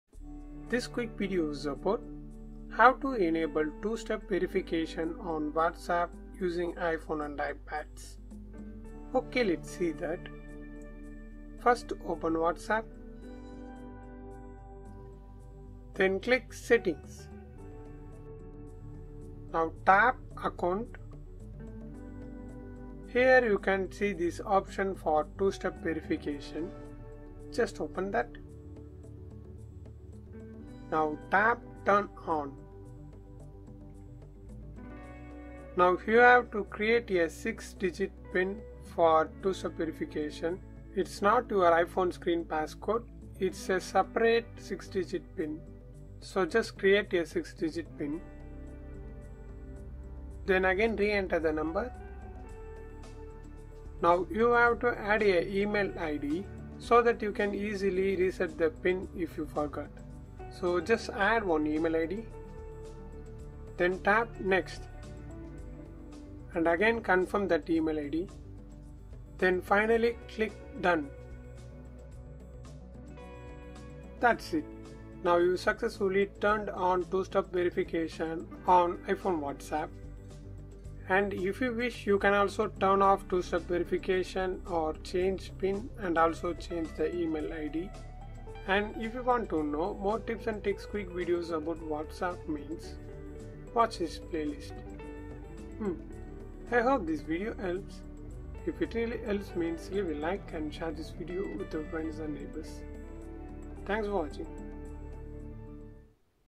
How to Enable Two Step Verification on Whatsapp iPhone?
How to Enable Two-Step Verification on Whatsapp iPhone? In this video, I show you how to turn on two-factor authentication on WhatsApp on iphone and ipad. Actually, You can easily enable two step verification on iphone whatsapp.